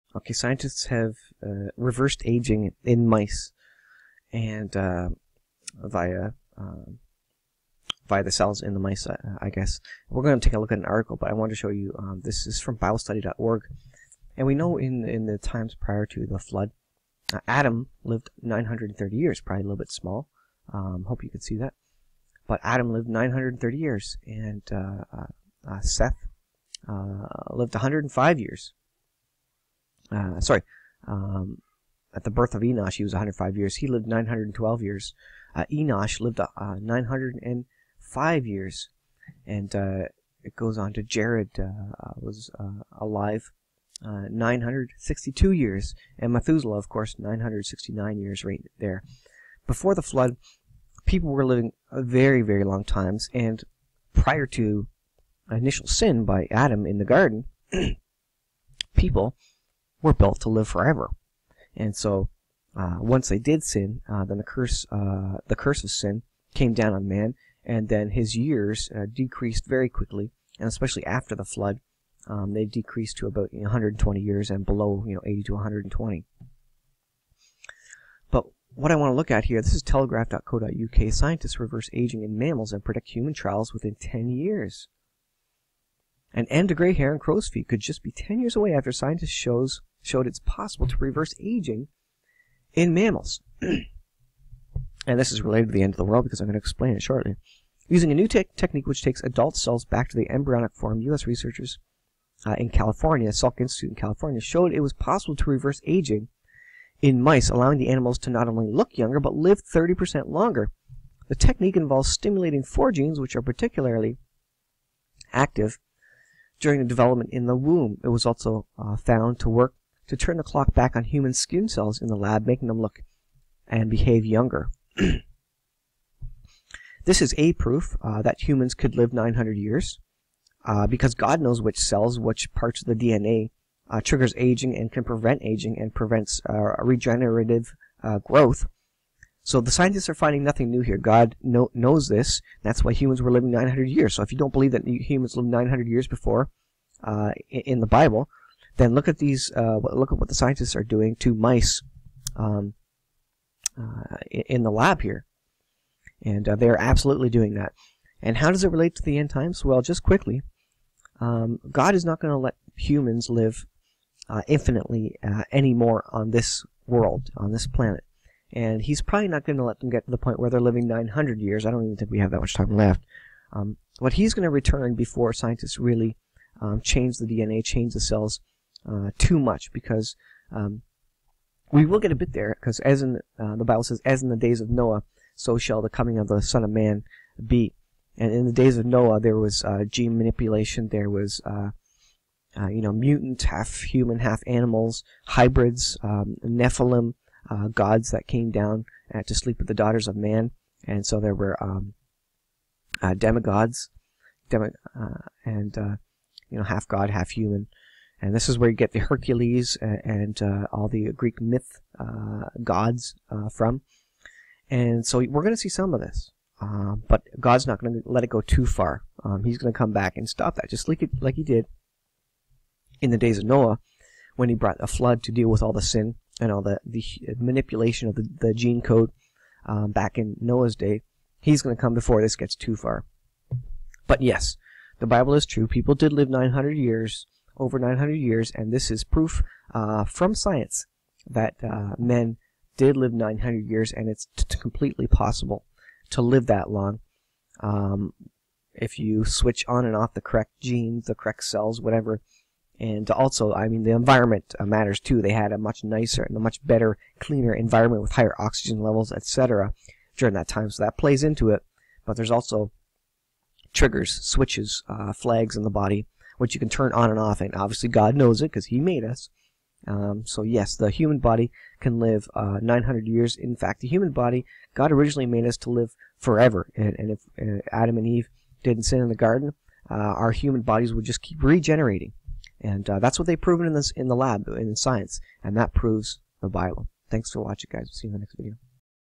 Science proves Humans can live 900 years just like Bible Records!
Science is coming around! Science figures out that there are switches in cells and DNA to tell them to continually regenerate.